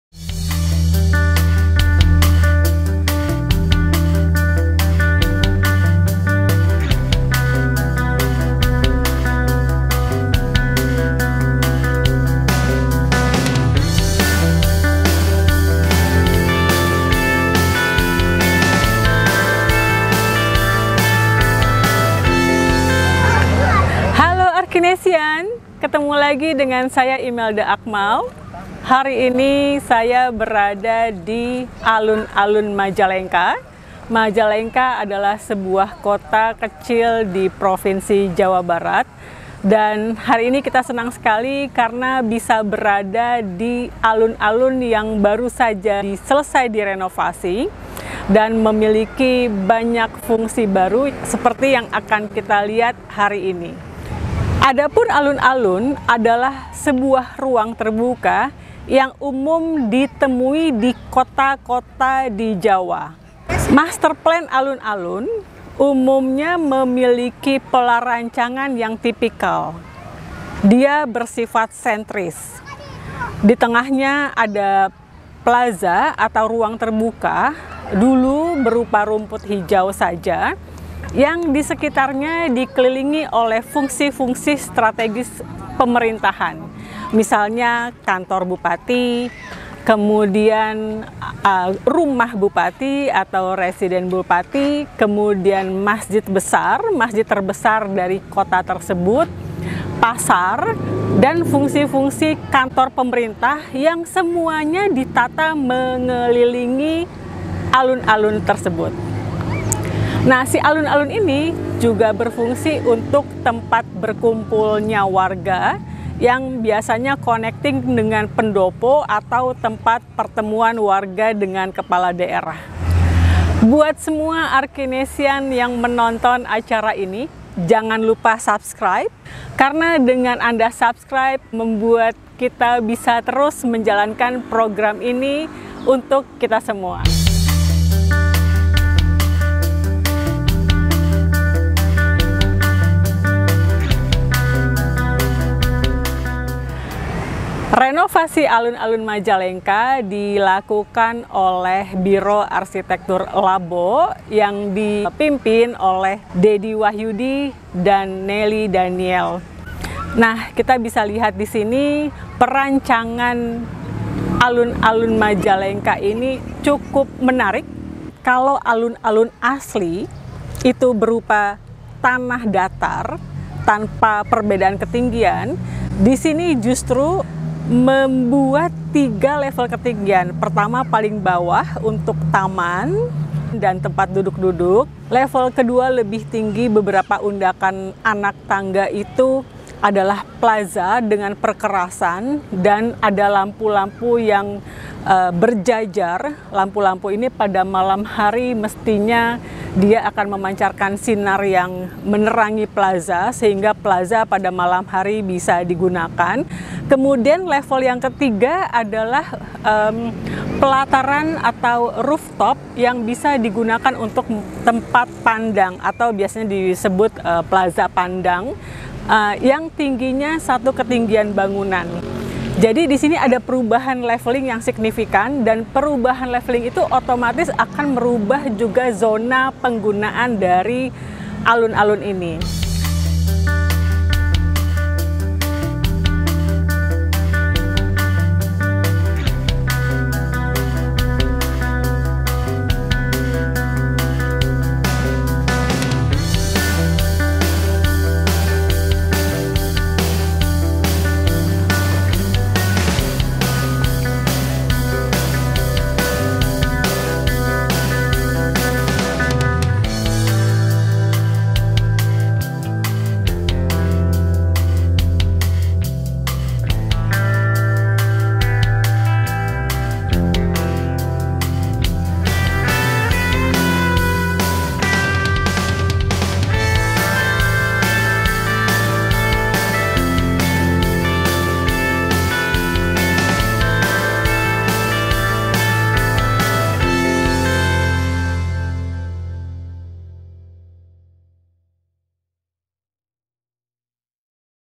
Alun Alun Majalengka
Majalengka memiliki alun alun menarik dengan 'tiga tingkat' berbeda yang didominasi terakota dan batubata. Biro arsitek LABO mendesain alun alun ini sehingga mempengaruhi wajah kota Majalengka menjadi tertata dan menghidupkan kembali material lokalnya.